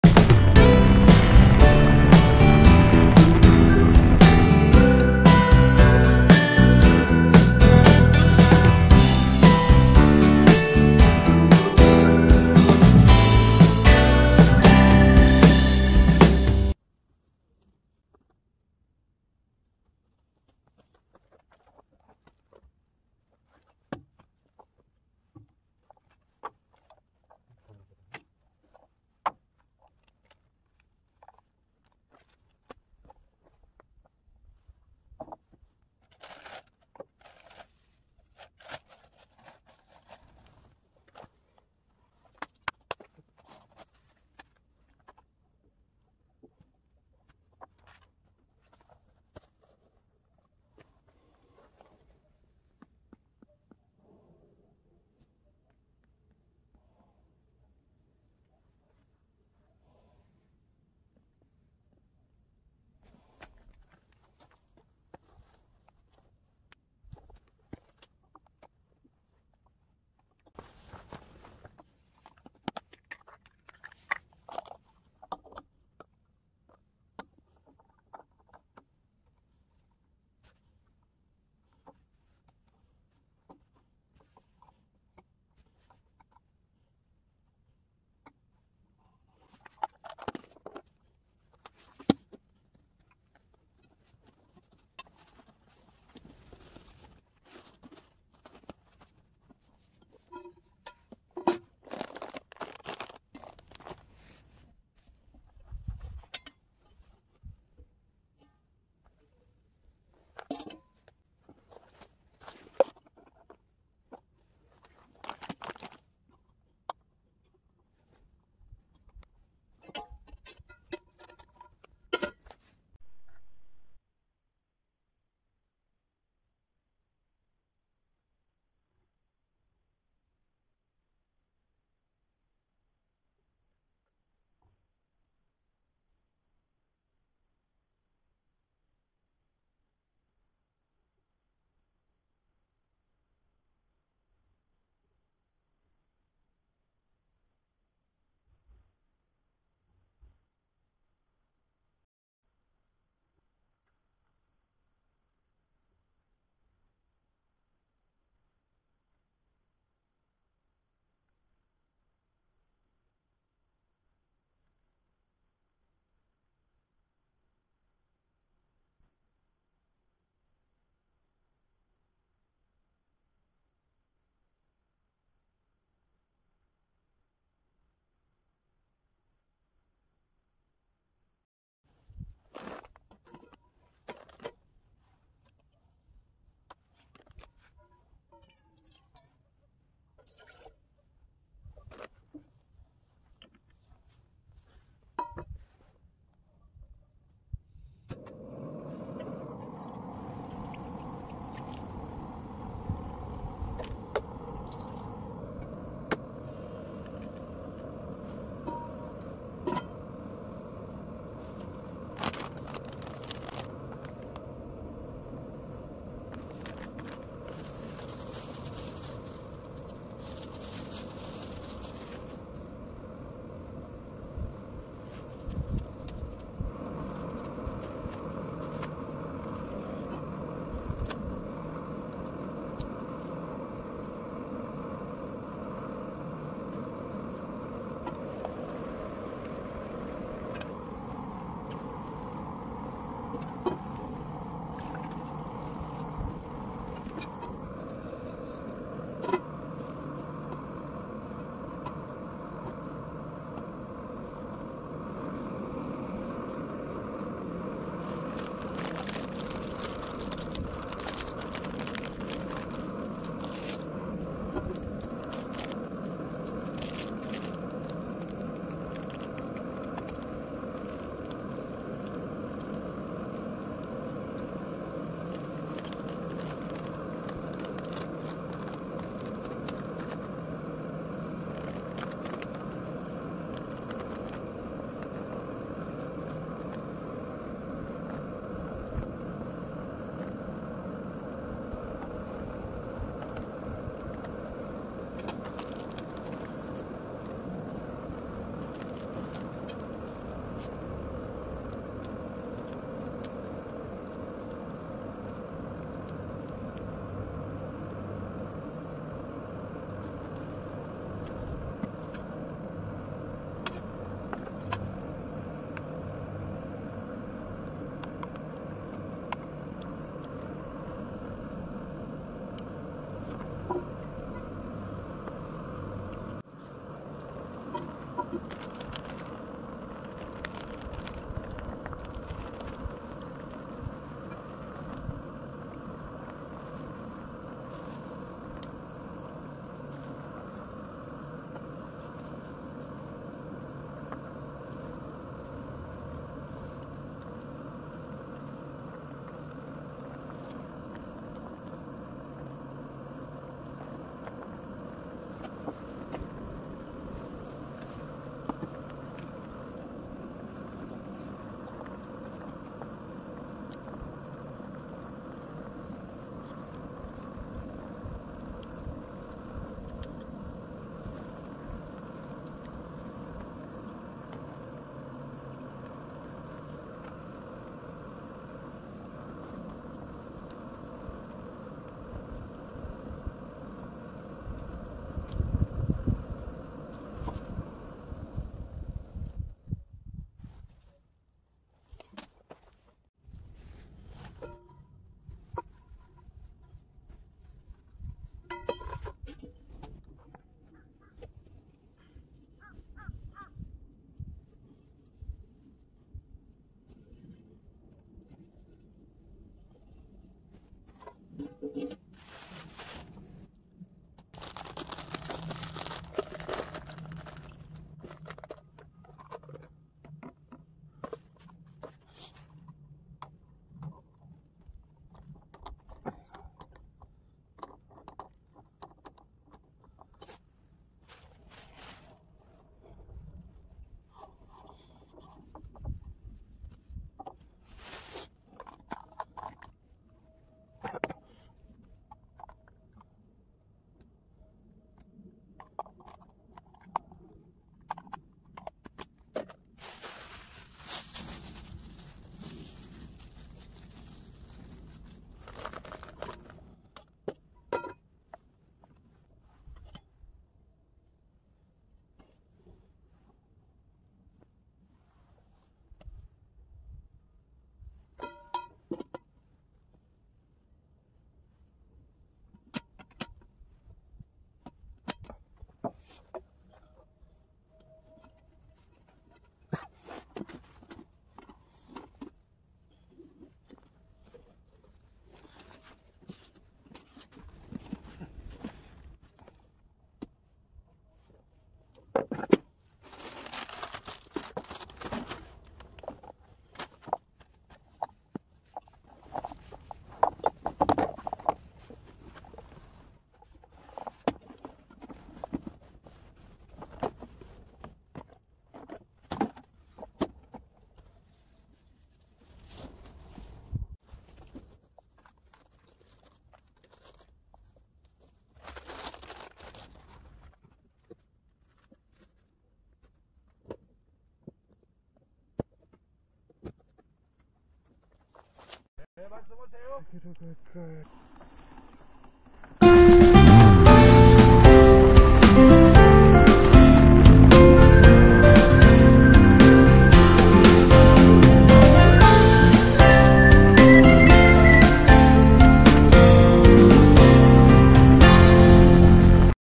Benelli 베넬리 TRK502X 20201124 점심한끼 by rockgo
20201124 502X 타고 망해산 임도길 점심한끼(어묵 라면)